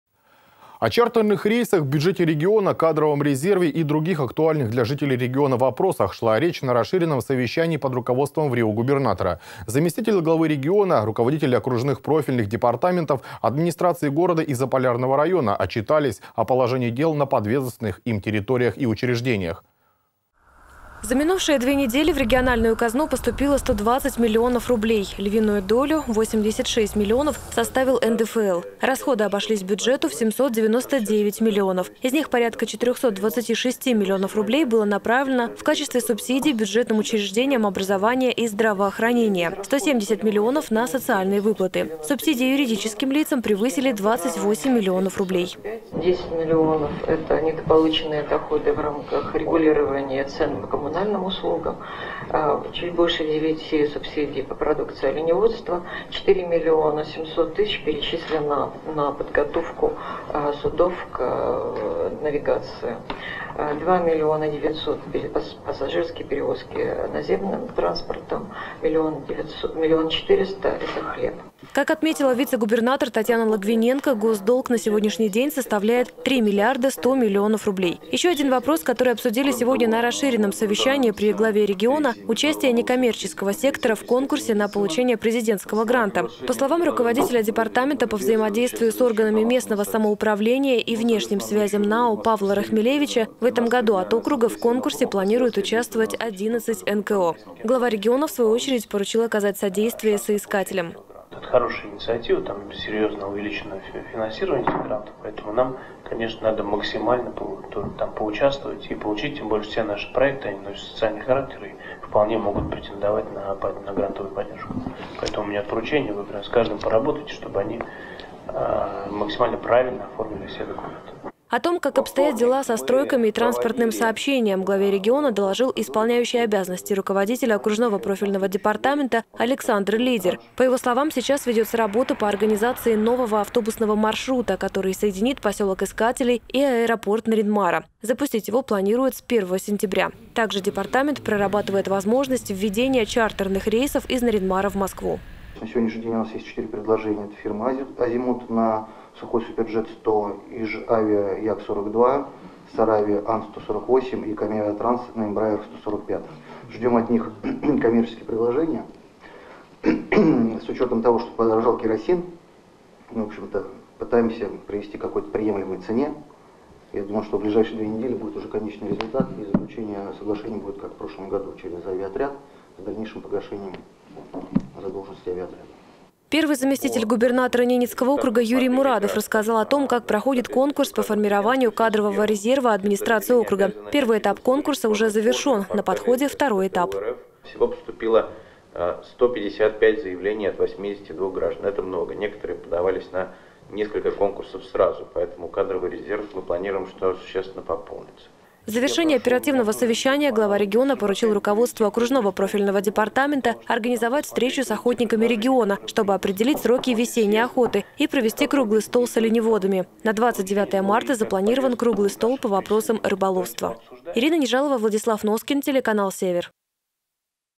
Четыре авиаперевозчика предлагают организовать сообщение по маршруту Нарьян-Мар — Москва
О чартерных рейсах, бюджете региона, кадровом резерве и других актуальных для жителей региона вопросах шла речь на расширенном совещании под руководством врио губернатора. Заместители главы региона, руководители окружных профильных департаментов, администраций города и Заполярного района отчитались о положении дел на подведомственных им территориях и учреждениях.